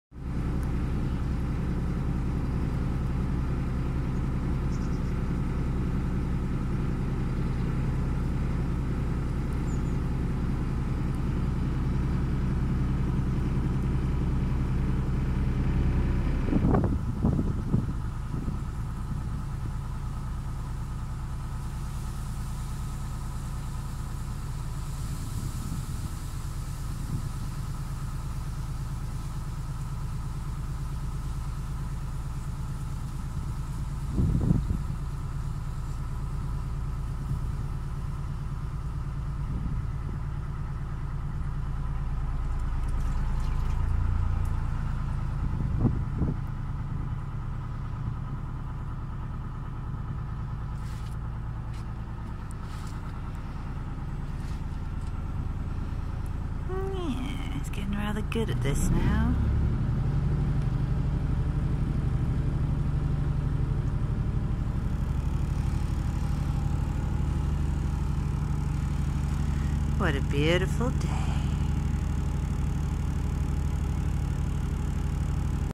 Rubble Queen 2017
Short clip cruising along the Stratford Canal and under Bridge 55